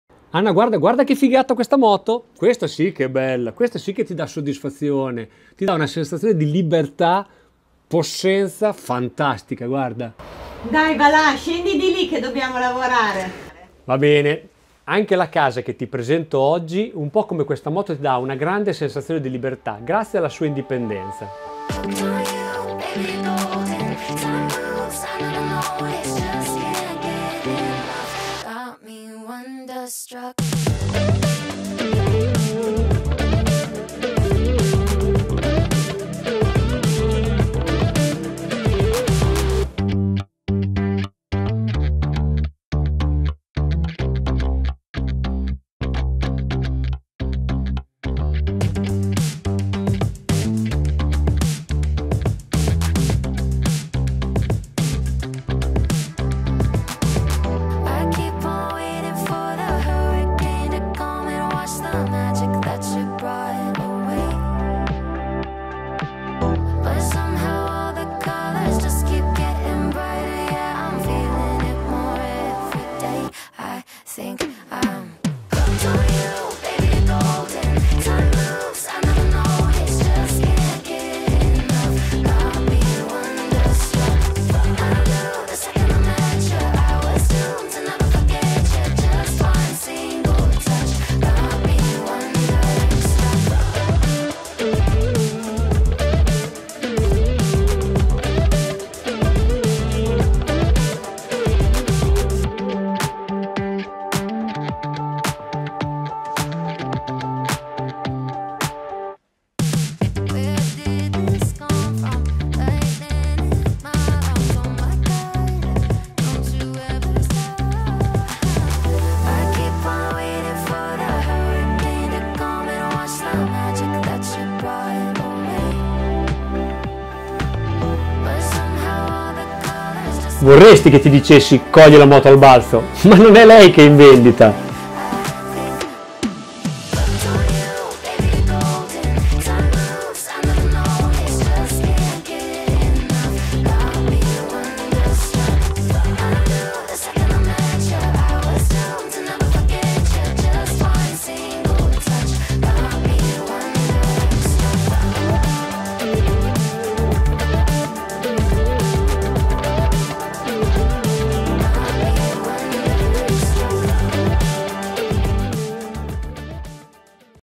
La libertà, simile a quella che potresti avere vivendo in una casa singola
L'ampio giardino sui tre lati, lo spazio per l'auto, per l'hobby del giardinaggio e gli spazi per mangiare all'esterno sono un punto di forza di questa soluzione. Anche le rifiniture danno un tocco di classe a questa casa che è vivibile fin da subito.
🎬 Guarda il video e se ti piace ciò che facciamo regalaci un like 👍, ci mettiamo tutto l'amore possibile per fare cose belle‼️